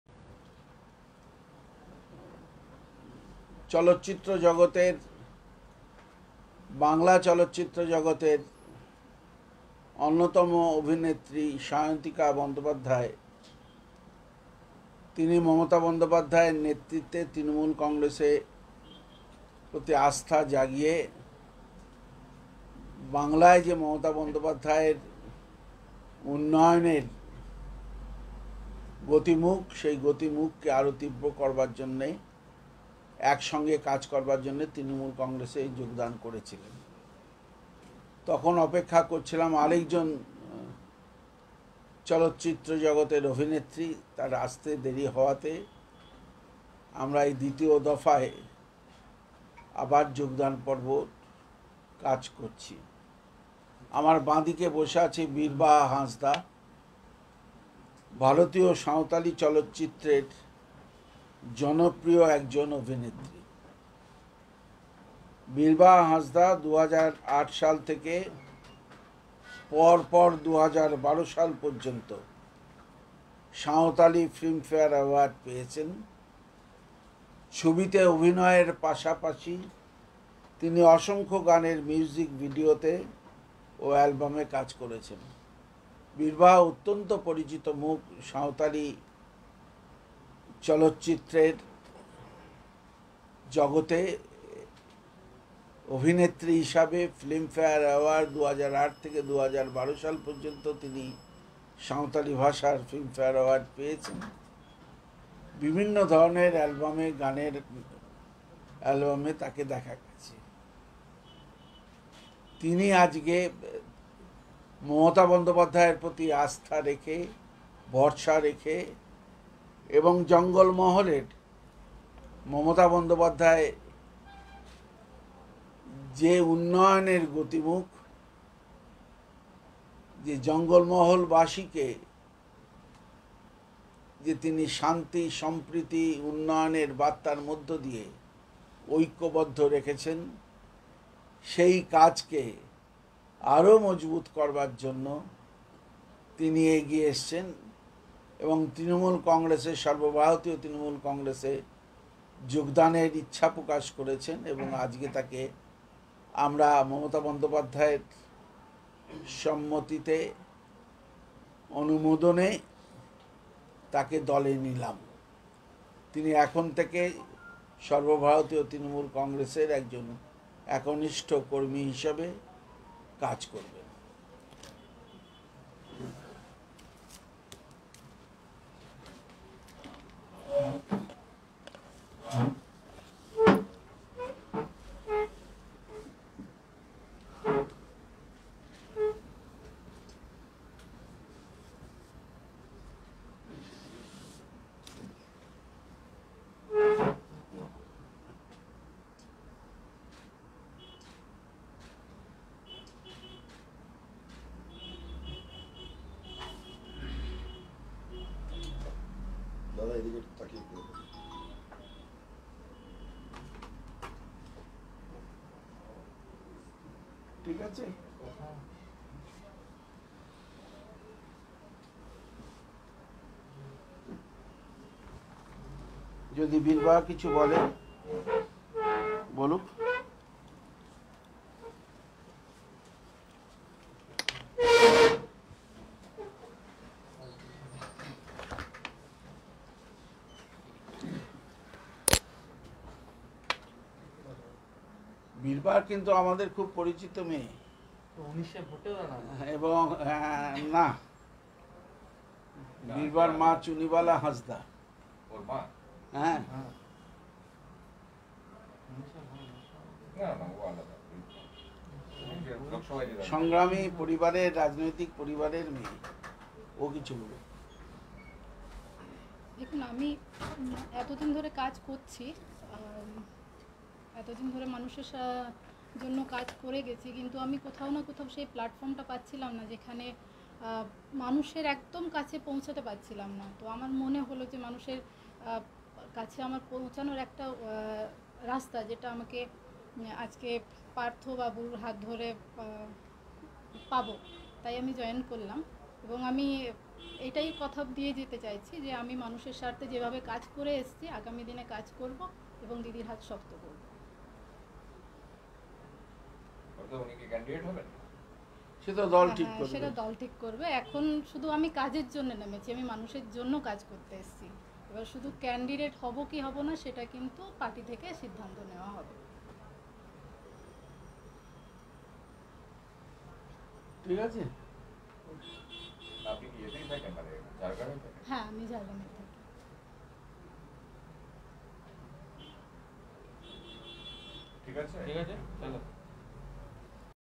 বিশিষ্ট সাঁওতালি অভিনেত্রী "বিরবা হাঁসদা"-তৃনমুল কংগ্রেস এ যোগদান করলেন|তৃনমুল ভবনে সাংবাদিক বৈঠকে
এই ভিডিওটি পশ্চিমবঙ্গের বিধানসভা নির্বাচন সম্পর্কে। অাজ পশ্চিবঙ্গের মাননীয় শিক্ষামন্ত্রী পার্থ চট্টোপাধ্যায়ের হাত ধরে সর্বভারতীয় তৃনমুল কংগ্রেস এ যোগদান করলেন বিশিষ্ট সাঁওতালি অভিনেত্রী "বিরবা হাঁসদা"

This video about West Bengal election. "Birba Hansda" Famous Saotali Actress she is joined on All India Trinamul Congress.